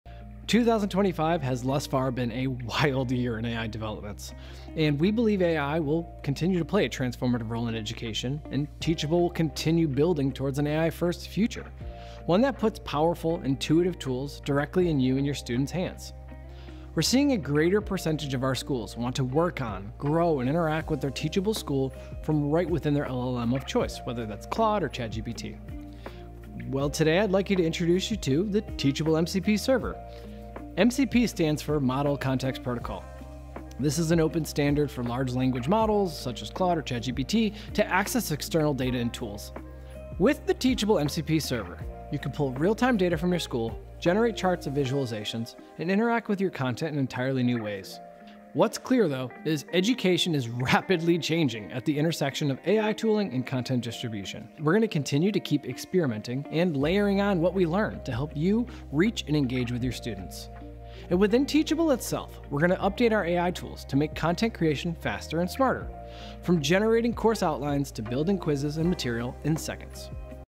Empowering educators with AI: Meet our new MCP server
2025 has been a wild year in AI development, and at Teachable, we’re committed to continuing to put the most powerful, intuitive AI tools in your hands 🤖

At Thrive 2025, our Head of Product, Aaron Roy, announced the release of Teachable’s MCP Server (Model Context Protocol), a major step forward in connecting your Teachable school directly with your favorite AI tools like Claude.

What this means: Real-time data for instant insights and dynamic exploration.